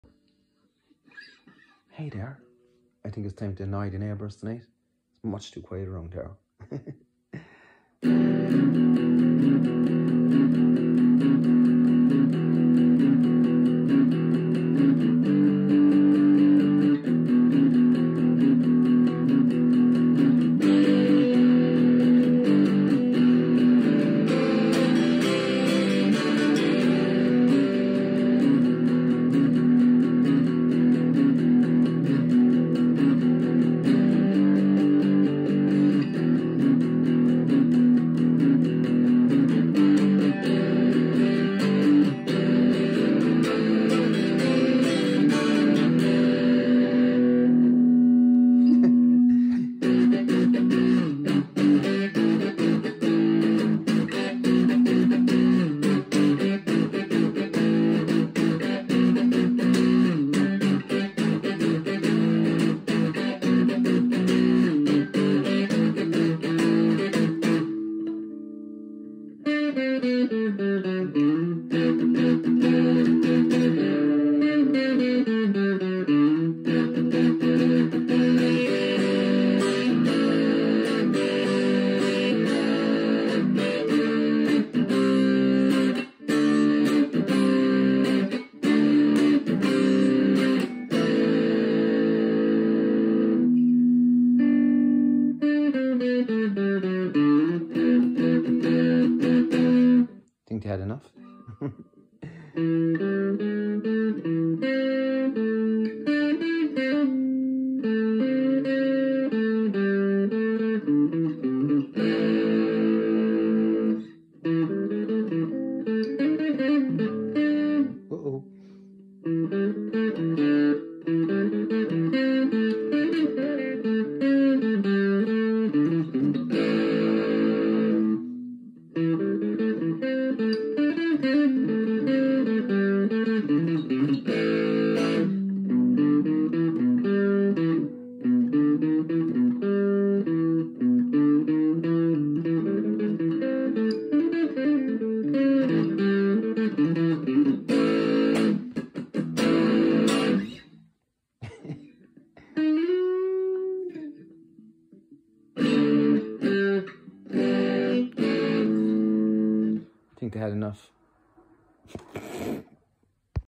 How To Make Your Neighbours MAD 😡 (guitar practice lol!) 🎸
Messing about keeping the neighbours awake! 🤪